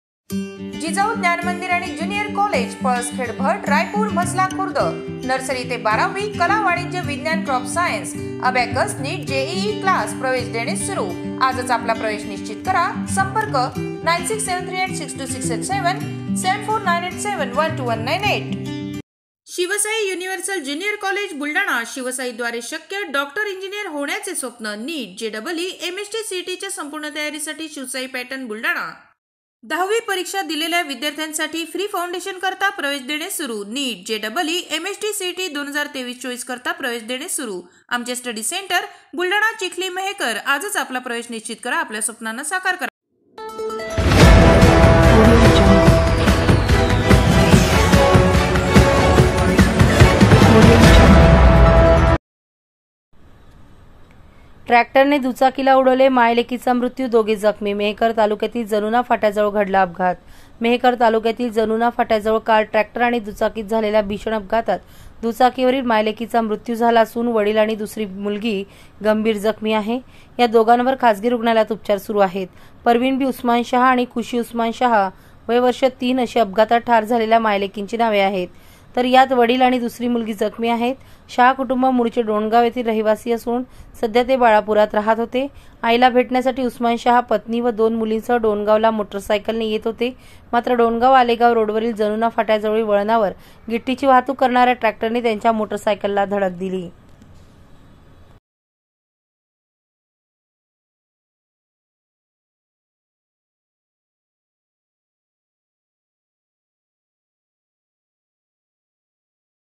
मेहकर : ट्रॅक्टरने दुचाकीला उडवले.. मायलेकीचा मृत्यू , दोघे जखमी..
मेहकर तालुक्यातील जनुना फाट्याजवळ घडला अपघात ..